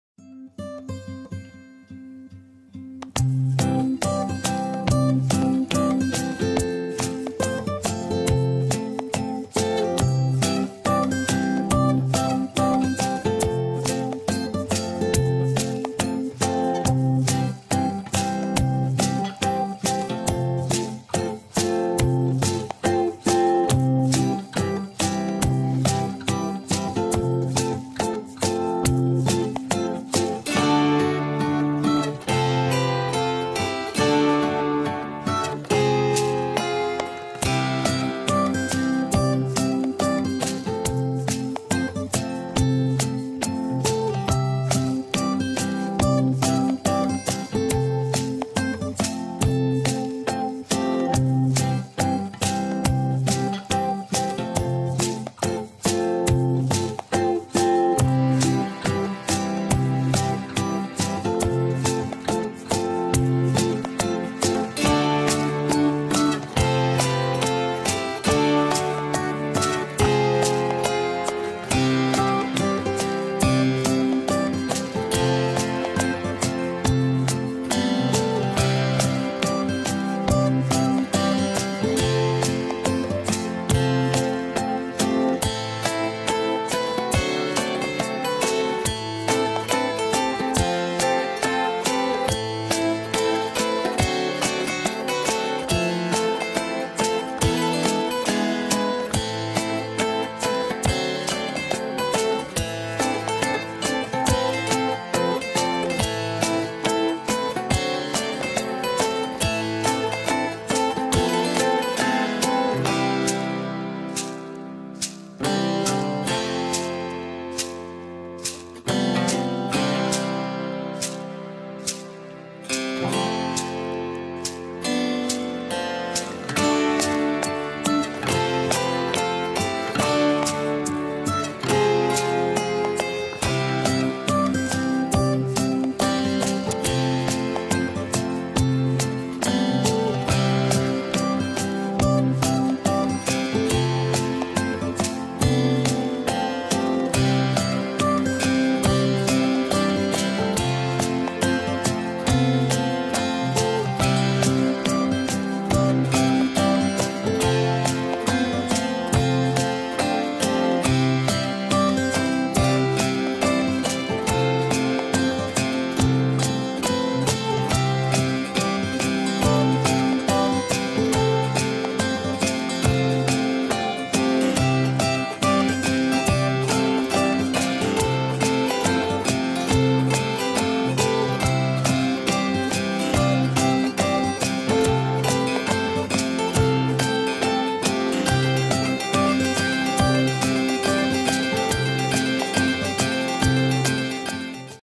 4. Italy · Busabout Italian Adventure June 2015
Checking out Pompeii, Sorrento, Capri and Amalfi

Footage captured with iPhone 6 camera and edited on iPhone 6.
Music, effects and grading all done on the 'Adobe Premier Clip’ app.